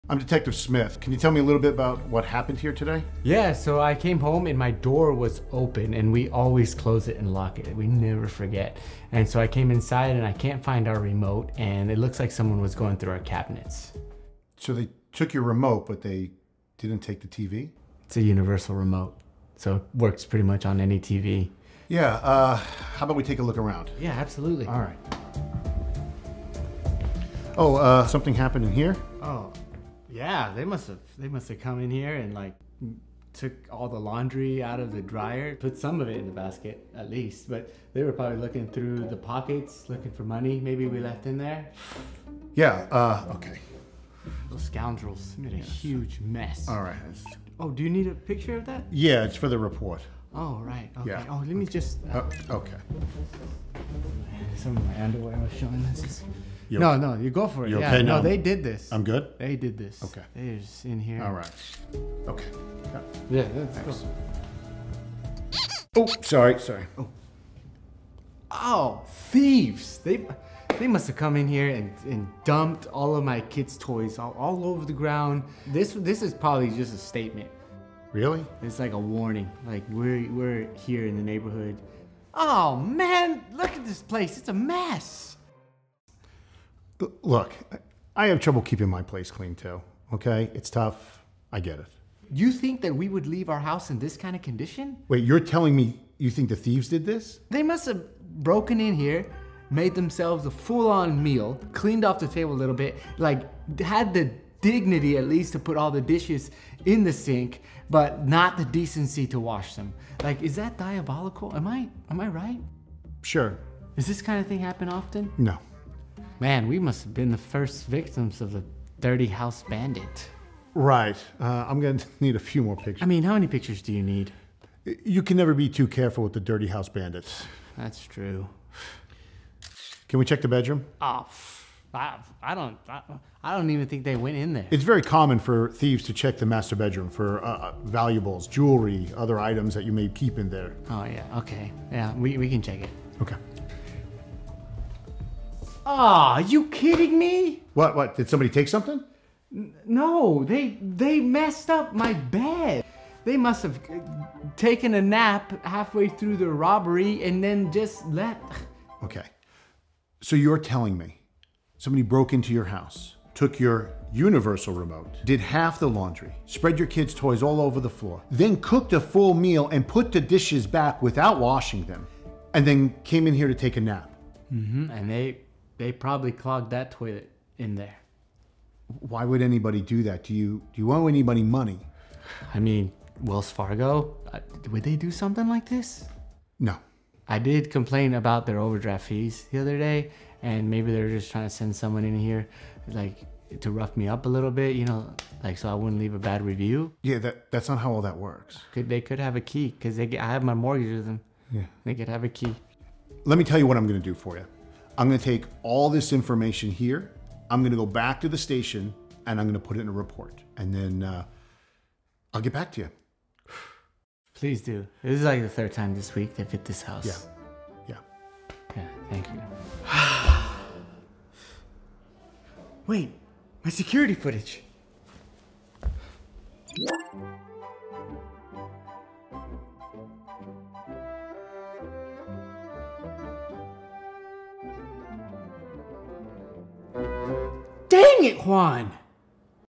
Dirty House Bandit | David Lopez
David Lopez calls a detective to solve his house robbery. The detective begins to investigate and soon finds out that David's situation is much more complicated than it seems. Will they find the thief or is it too late?!